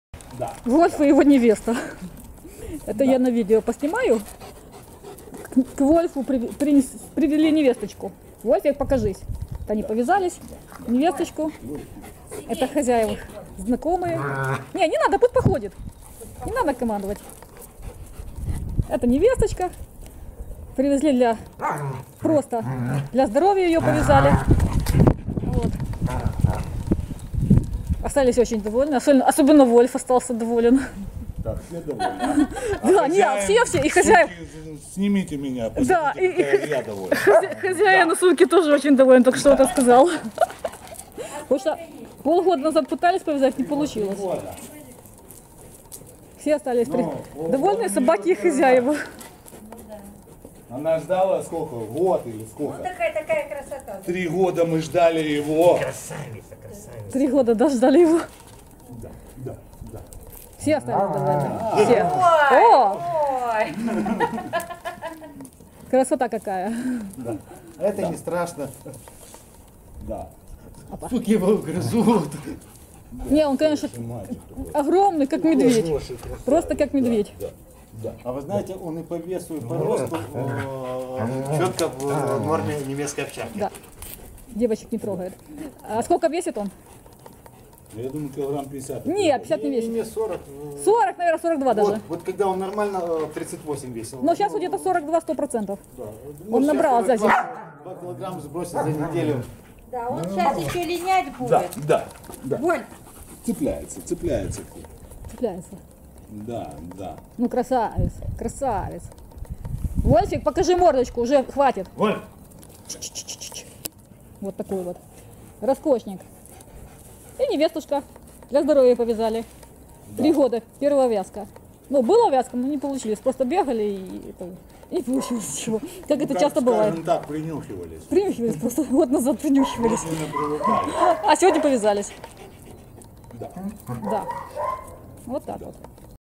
Немецкая овчарка Вольф и его невеста.Одесса.
Свадьба состоялась.Все остались довольны.